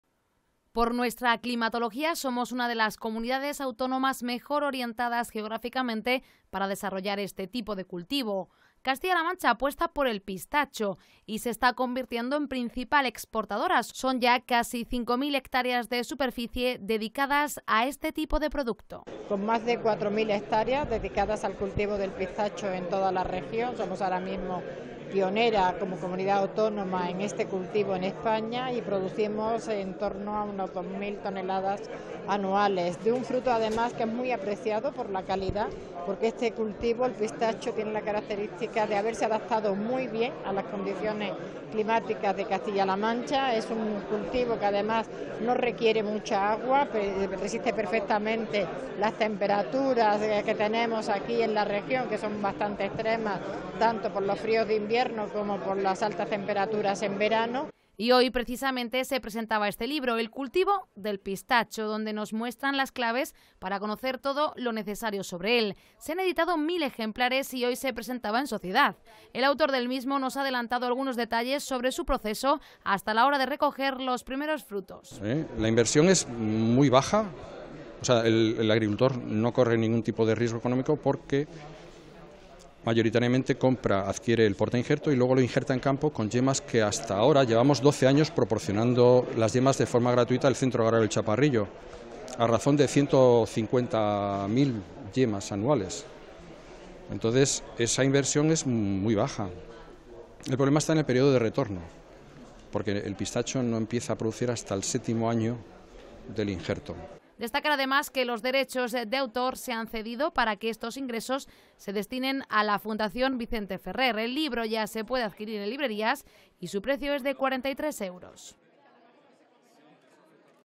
1.000 ejemplares se han editado del libro " El Cultivo del Pistacho"
1.000 ejemplares se han editado del libro " El Cultivo del Pistacho" y es que Castilla-La Mancha ya cuenta con casi 5.000 hectáreas dedicadas a este cultivo y se está convirtiendo en una gran exportadora. En este libro se explican las claves para conocer todos los detalles de este árbol leñoso.
Por nuestra climatología somos una de las comunidades mejor orientadas geográficamente para desarrollar este tipo de cultivo. Castilla-La Mancha apuesta por el pistacho y su cultivo y se está convirtiendo en principal exportadora son ya casi 5.000 hectáreas de superficies dedicadas al cultivo de este producto.
Y hoy precisamente se presentaba este libro "El Cultivo del Pistacho" donde nos muestran las claves para conocer todo lo necesario sobre él. Se han editado 1.000 ejemplares y hoy se presentaba en sociedad. El autor del mismo nos ha adelantado algunos detalles sobre su proceso hasta la hora de recoger los primeros frutos.
Destacar además que los derechos de autor se han cedido para que estos ingresos se destinen a la Fundación Vicente Ferrer, el libro ya se puede adquirir en librerías y su precio es de 43 euros.